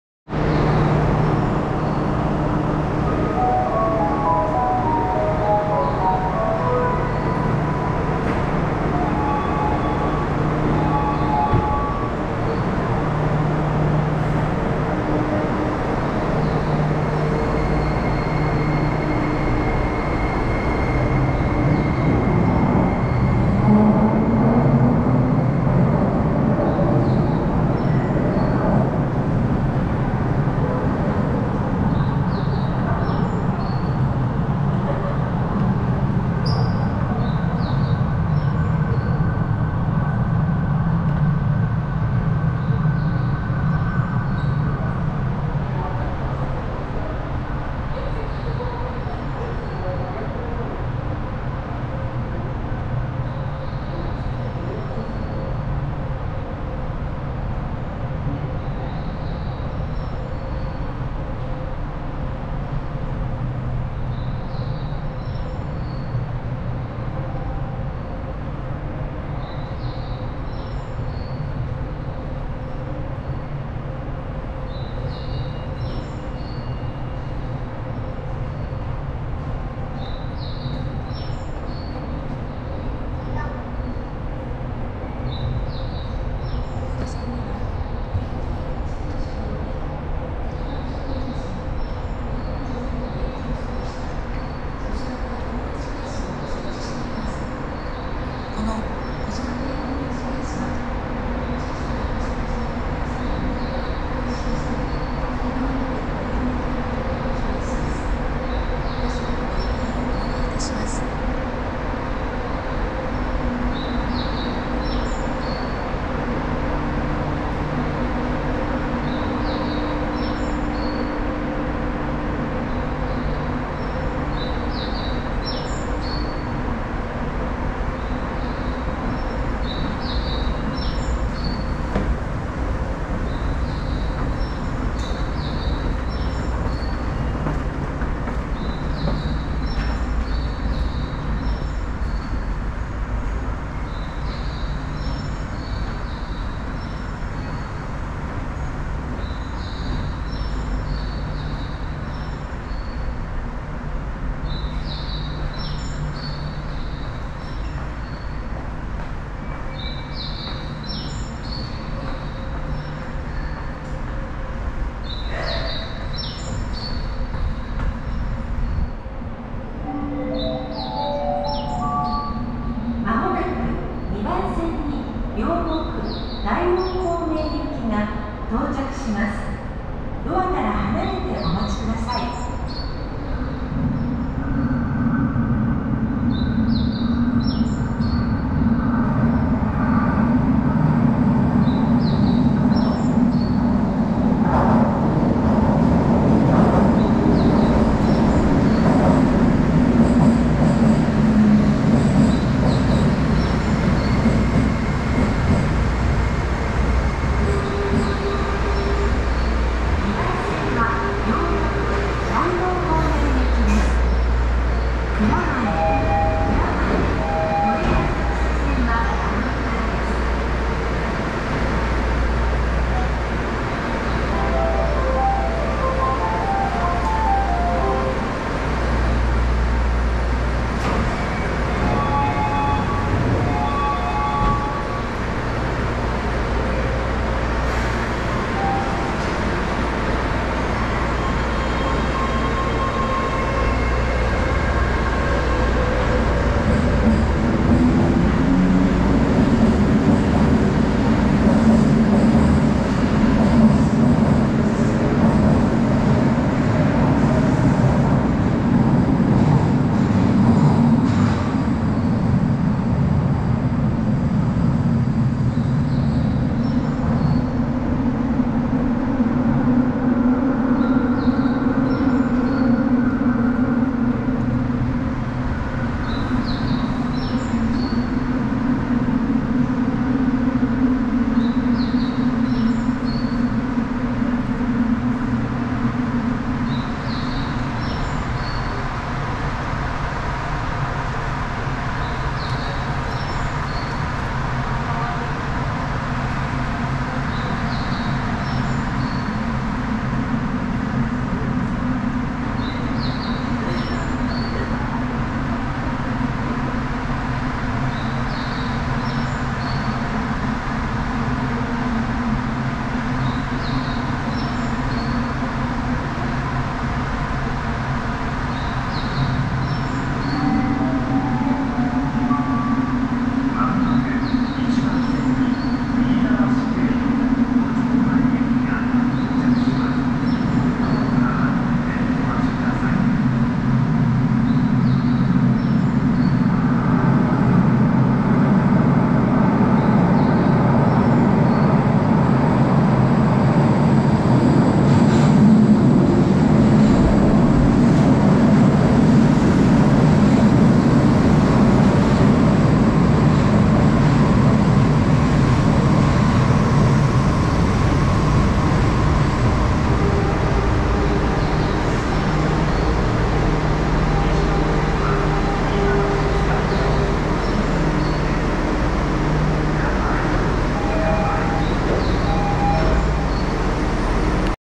【4K】Kuramae Station E11 蔵前駅 - Toei Ōedo Line Platform Walk【JAPAN】
Just my personal video recorded while I was exploring Tokyo, Japan.
I would like to share the authentic video about Tokyo, Japan. So I recorded this video in 4K quality by my GoPro and uploaded it directly without any editing.

◇ Tokyo Japan Real Estate
- Are you finding your dream home in Tokyo, Japan?